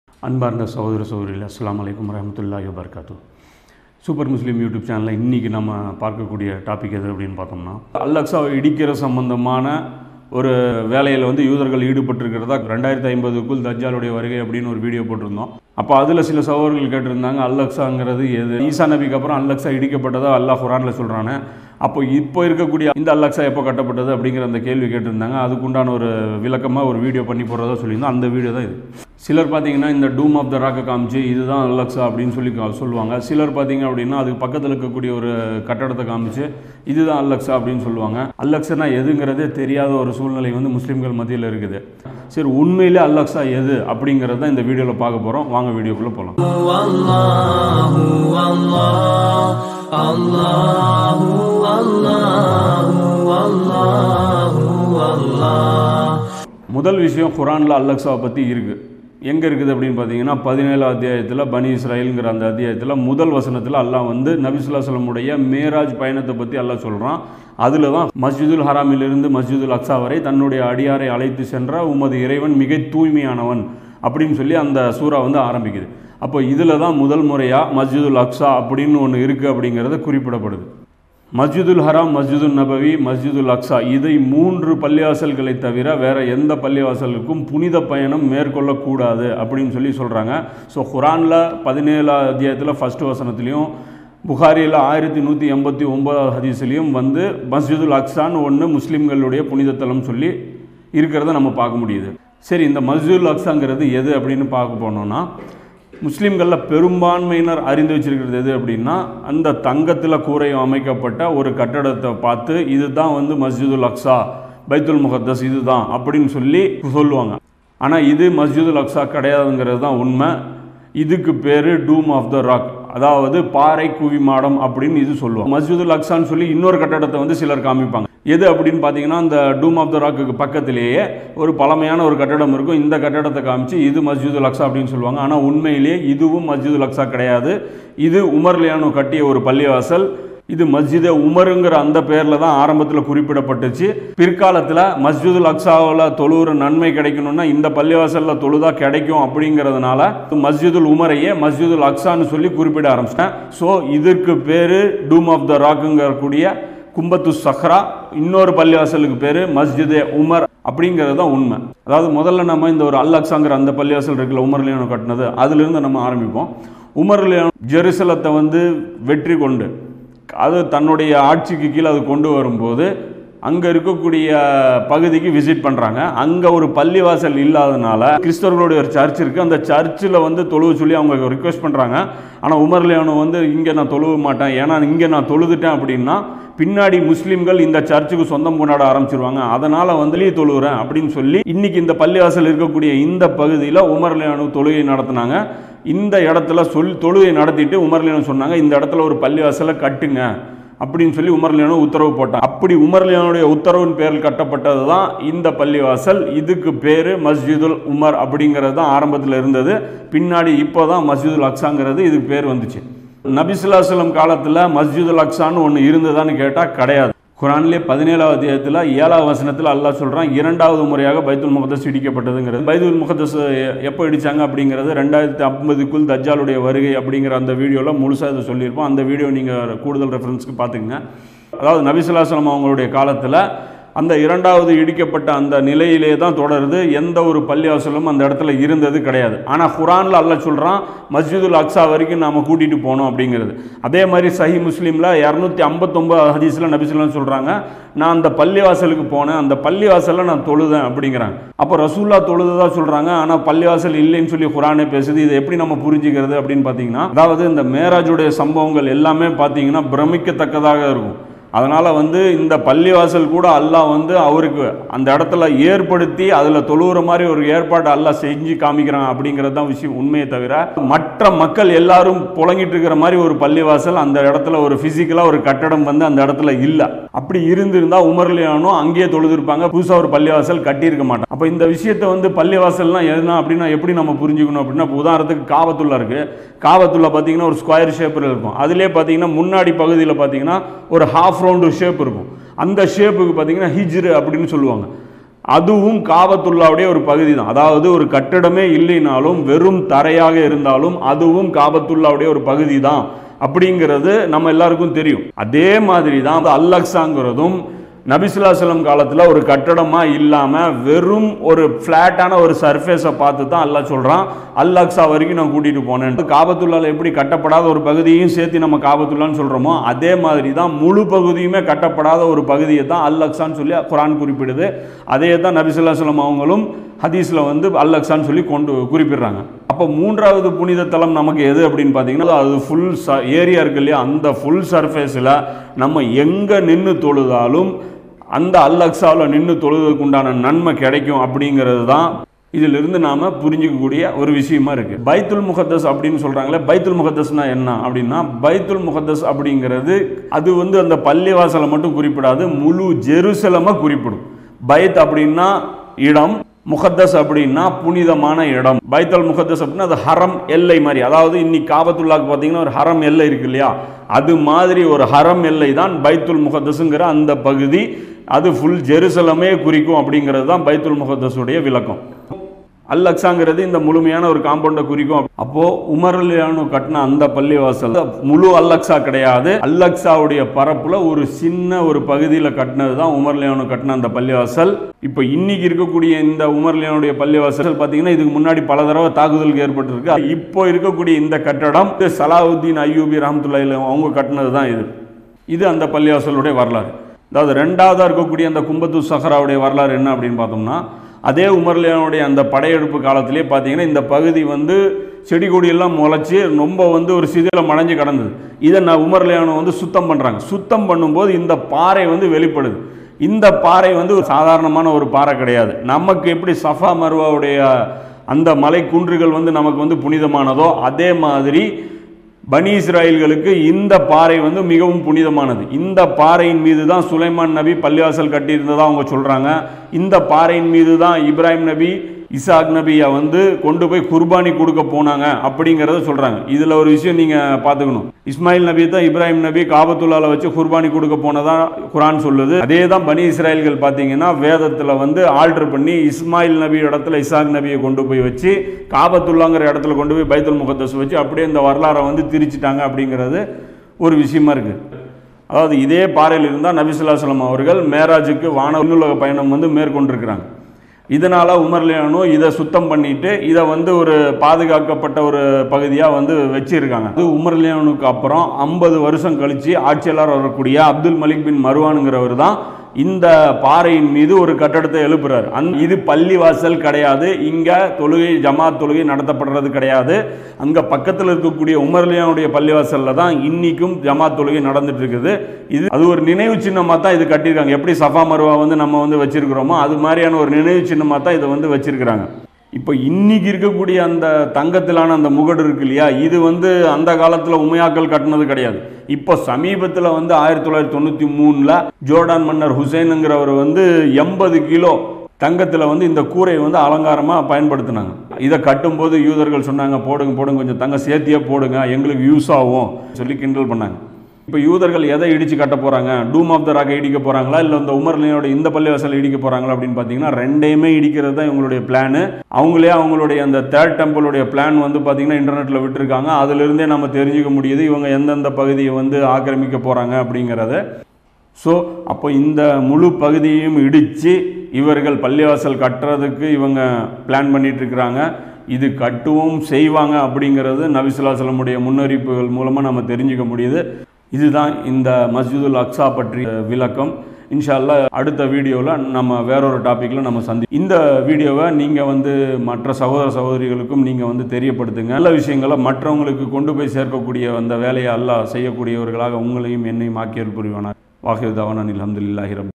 SM4 | உண்மையான அல் அக்ஸா எது?தெரியுமா?
முஸ்லிம்களின் மூன்றாவது புனித தளமான மஸ்ஜிதுல் அக்ஸா எது என்று பெரும்பான்மை மக்களுக்கு தெரியாது. இது பற்றிய சுருக்கமான வரலாறு புகைப்படங்களுடன் விளக்கப்பட்டுள்ளது# பைத்துல்முகத்தஸ்bayan